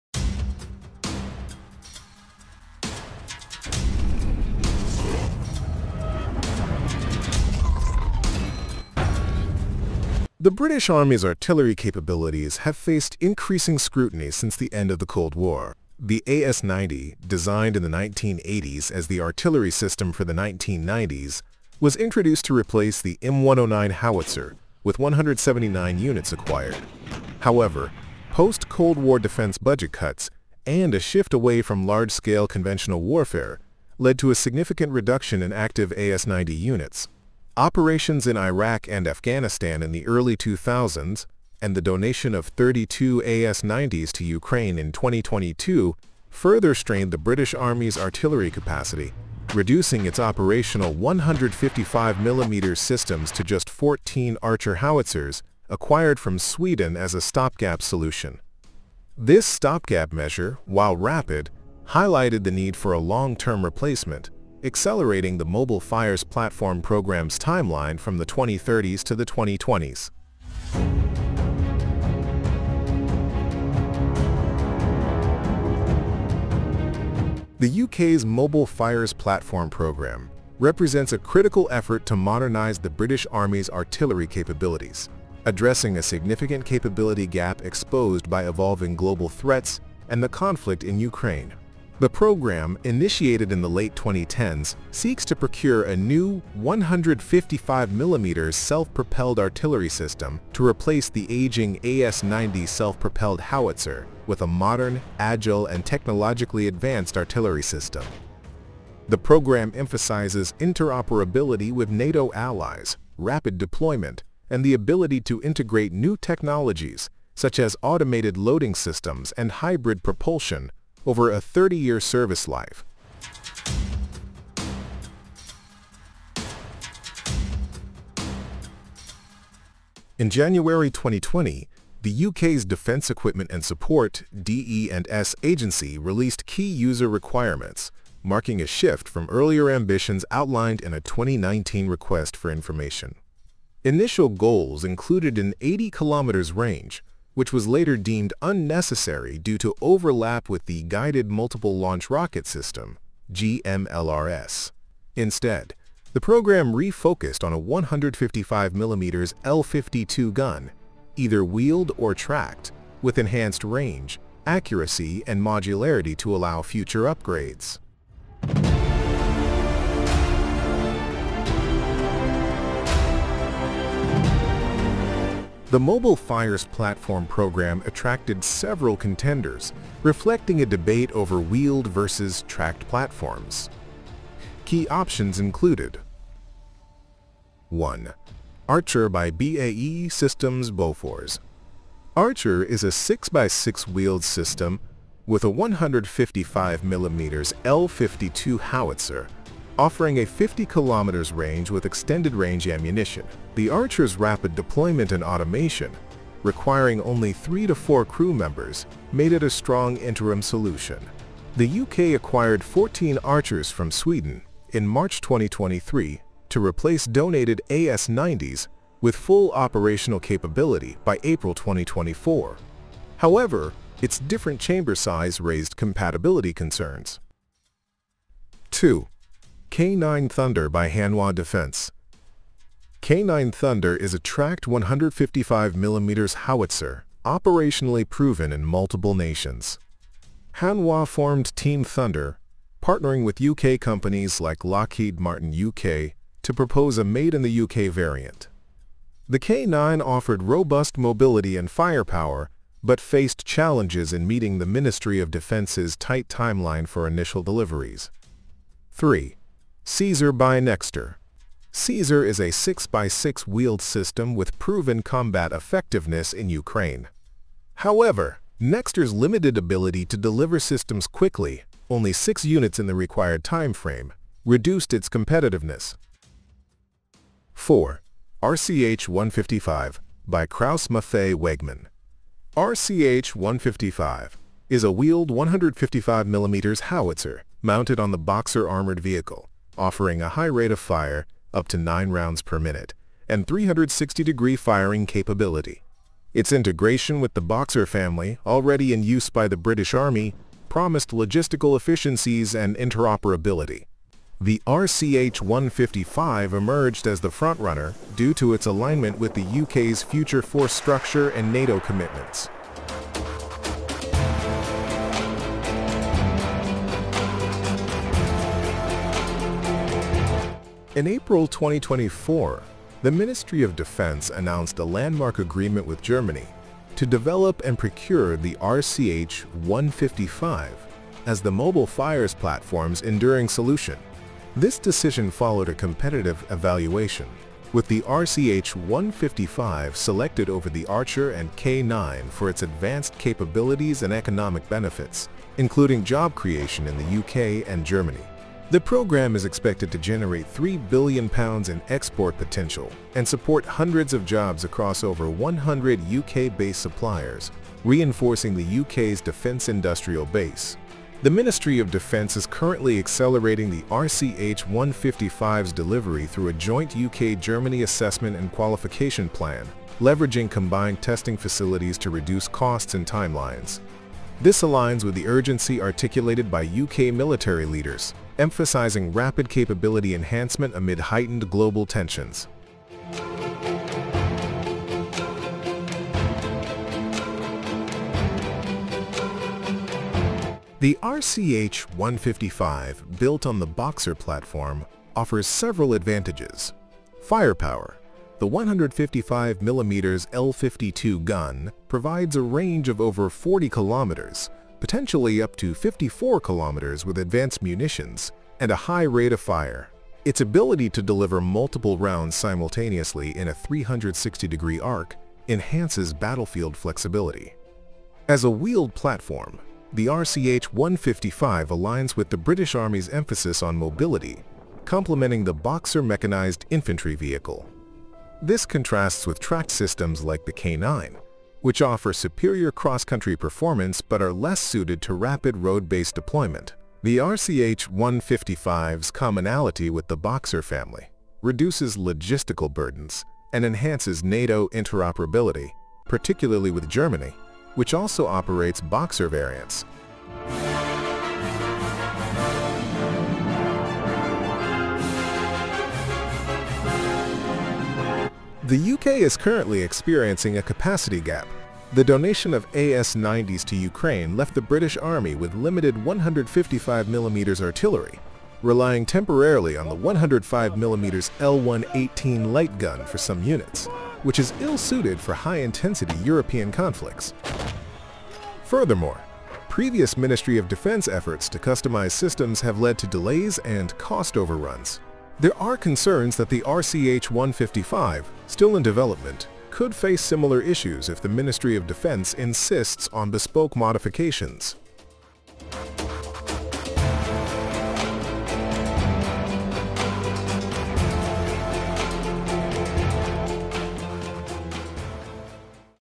Here Are The Main Reasons Why The UK Chose The RCH 155, Over K9, Caesar, Archer Howitzer
In January 2020, the UK’s DE&S agency released key user requirements. Initial goals included an 80km range, which was later deemed unnecessary due to overlap with the Guided Multiple Launch Rocket System. Instead, the programme refocused on a 155mm L52 gun, either wheeled or tracked, with enhanced range, accuracy, and modularity to allow future upgrades.

The Mobile Fires Platform programme attracted several contenders, reflecting a debate over wheeled versus tracked platforms. Key options included:
Archer 8x8 (BAE Systems Bofors)
Caesar 6x6 (Nexter)
K9 Thunder Tracked (Hanwha Defense)
RCH 155 6x6 (KMW)

Of all these candidates, the RCH 155 was chosen as the UK's Mobile Fires Platform.
So, why did the UK choose the RCH 155? Watch this video until the end to find out.